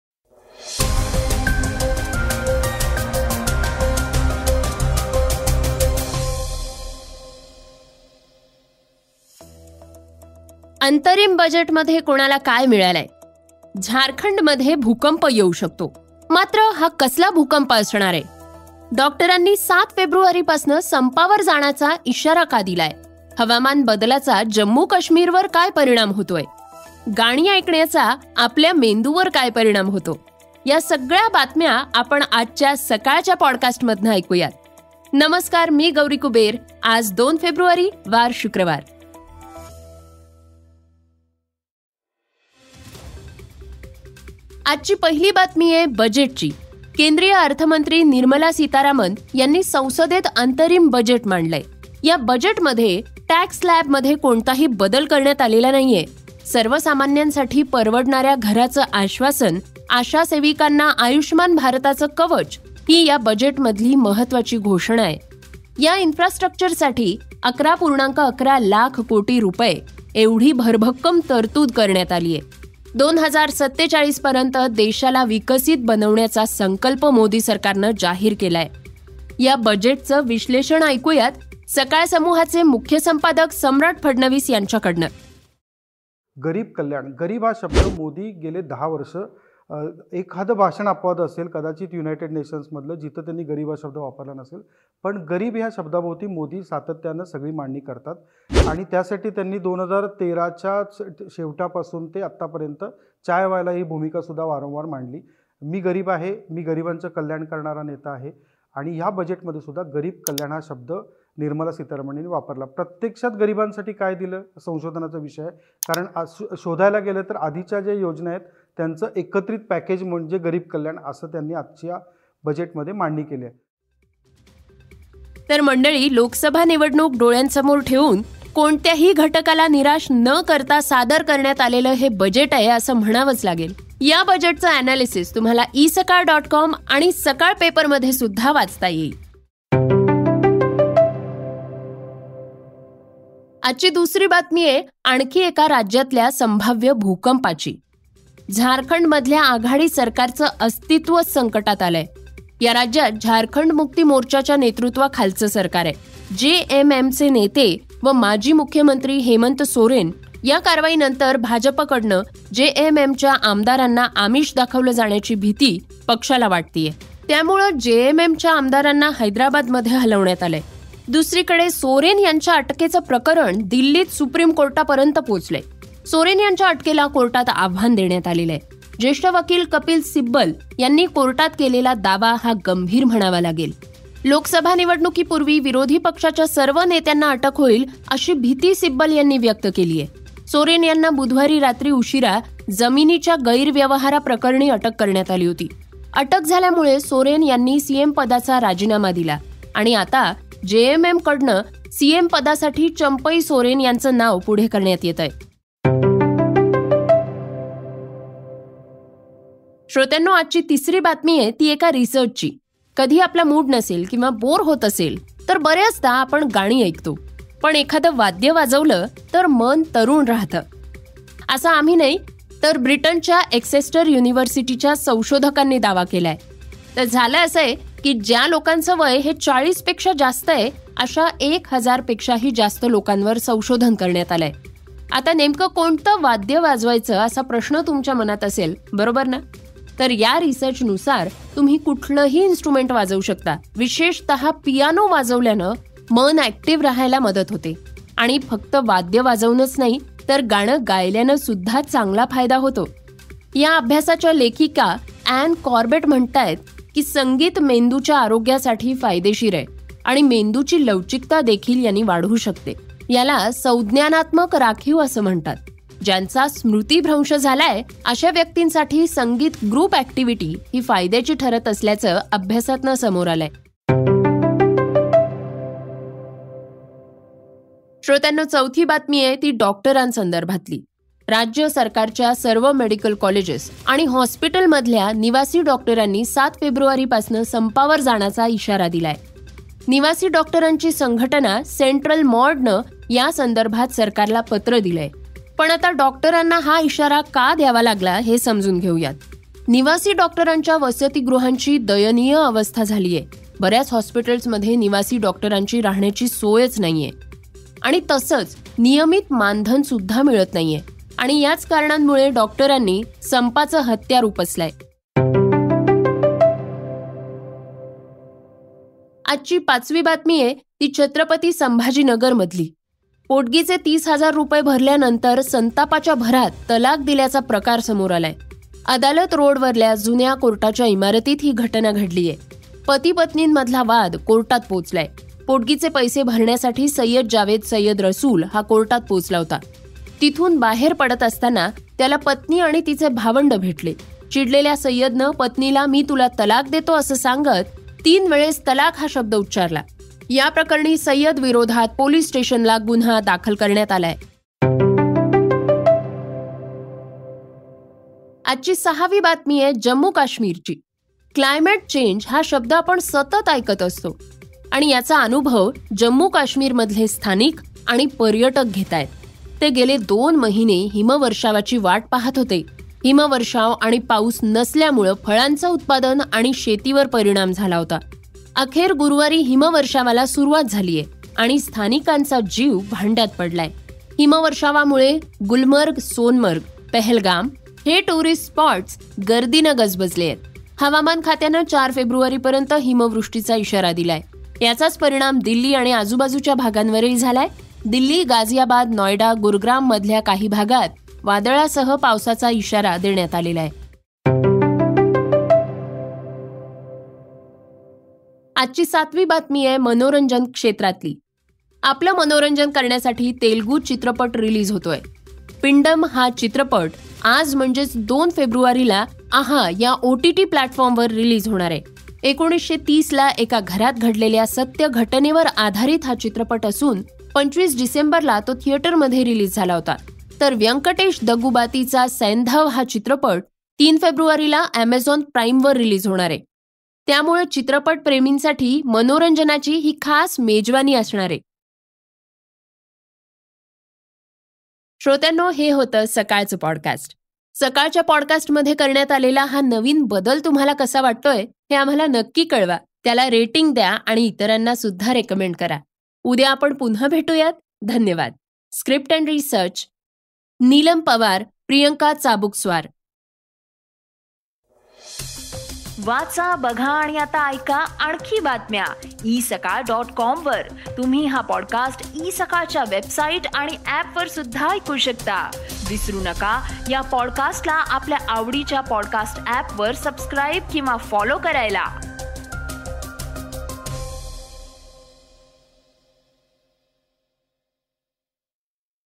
बजेट Explained ते कोर्टाच्या आवारातच पत्नीला दिला तलाक Marathi News Podcast
1. अंतरिम बजेटमधून मिळालं काय?
2.  झारखंडमधील आघाडी सरकारचं अस्तित्व संकटात
3. गाण्यांचा मेंदूवर सकारात्मक परिणाम
4.  डॉक्टरांनी दिला ७ फेब्रुवारीपासून संपावर जाण्याचा इशारा
5. कोर्टाच्या आवारातच पत्नीला दिला तलाक
6. हवामानबदलाचा जम्मू-काश्मिरवर परिणाम
7. तेलुगु चित्रपटांच्या रुपात चाहत्यांना मिळणार मनोरंजनाची मेजवाणी
स्क्रीप्ट अँड रिसर्च - निलम पवार, प्रियंका चाबूकस्वार